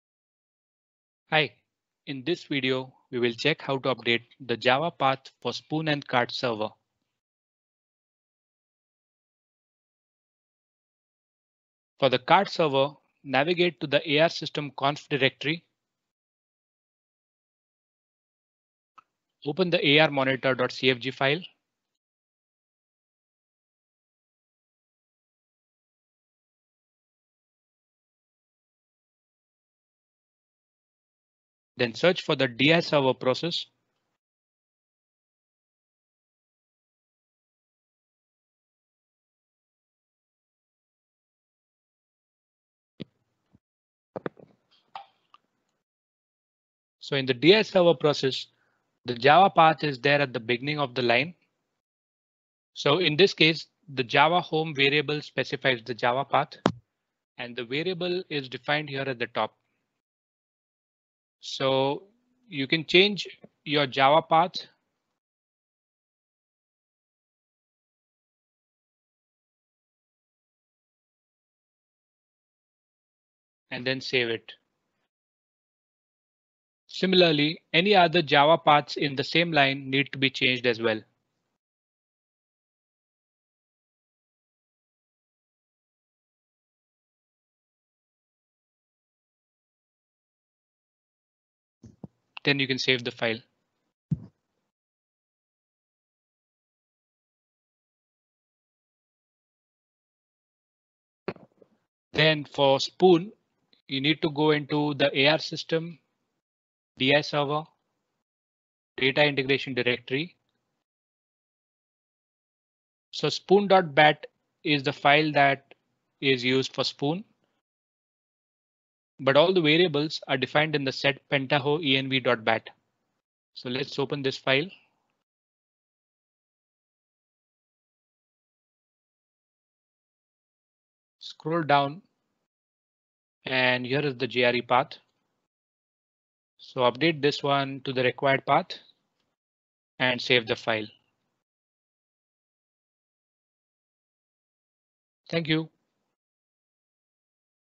BMC Helix CMDB: How to update the JAVA path for Spoon and Carte Server
Provide help when updating the JAVA path for Spoon and Carte Server